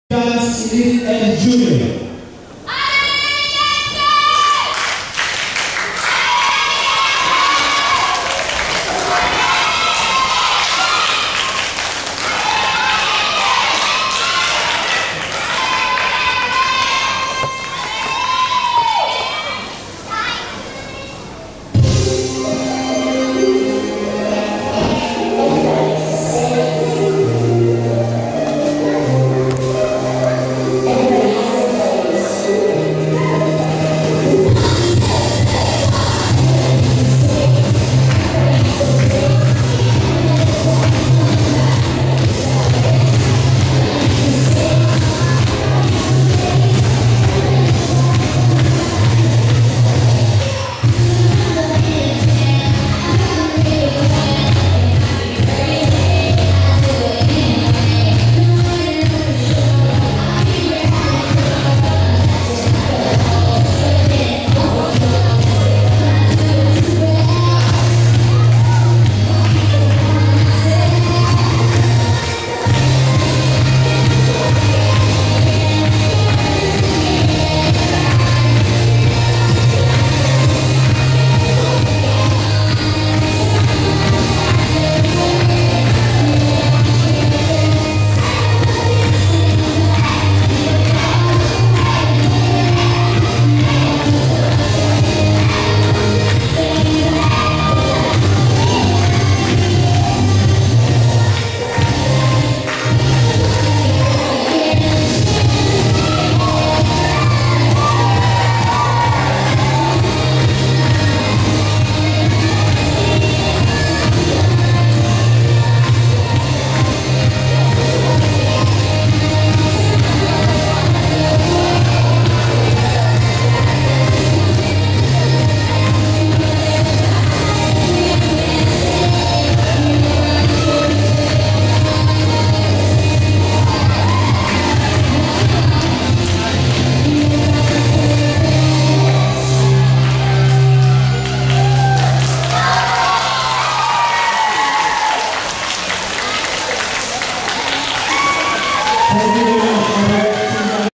IDSU Asia Dance Sport Cup 2018 - 02 ALBIREX Team Dance Elite & Jr - Freestyle Pom Jun./P.S. Small
International Dance Sport Union Asia Dance Sport Cup 2018
ALBIREX Team Dance Elite & Jr (Singapore)
Freestyle Pom Junior Primary School Small